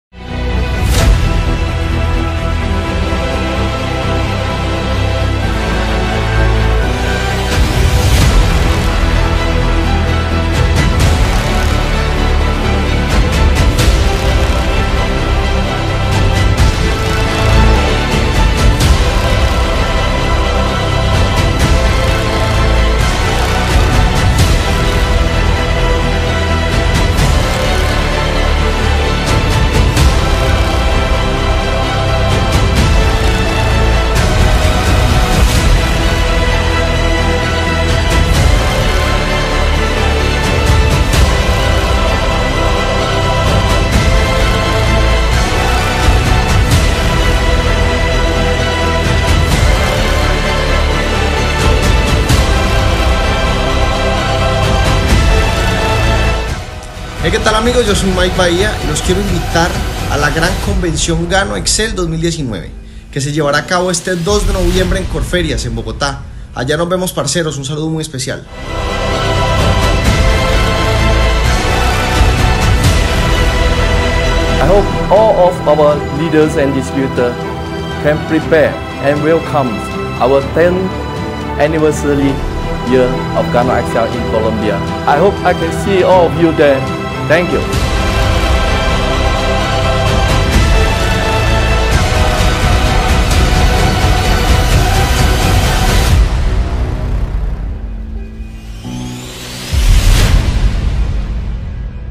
¡Faltan pocos días para nuestra Gran Convención Gano Excel 2019!
¡No te puedes perder la oportunidad única de hacer parte la celebración de nuestros primeros 10 años!

Tendremos grandes sorpresas para tí, recibe entrenamientos de altísimo nivel de la mano de nuestros Diamantes Royales,  Vive momentos únicos en compañía de nuestro Presidente y Fundador Mr. Leow Soon Seng, disfruta de nuestro concierto de cierre con Mike Bahía y obtén nuestro mug conmemorativo Gano Excel 10 años, exclusivo para nuestros asistentes.

¡No te quedes sin tu boleta! ven y celebra en familia nuestros primeros 10 años.